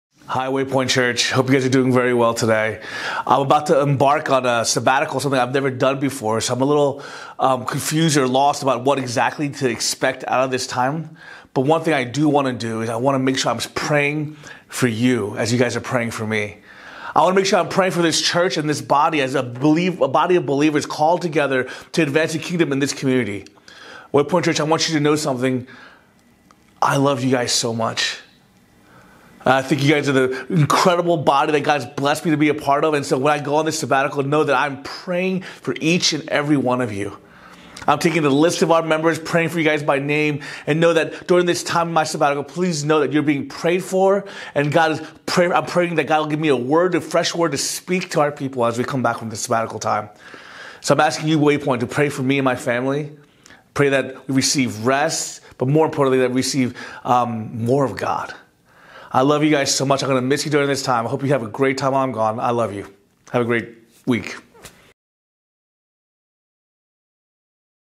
Mid-Week Encouragement 5/12/21
Pastor Lawrence shares a brief word of encouragement and a prayer request as he prepares for his upcoming sabbatical.